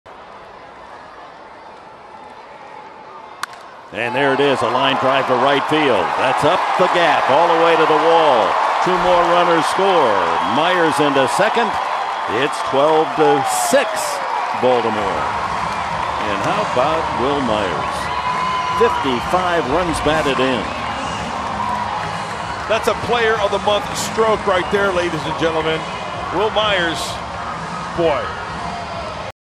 BAL@SD: Myers gaps a double to plate two runs
Wil Myers belts a double to the wall in right-center field, plating Alex Dickerson and Alexei Ramirez to cut the deficit to 12-6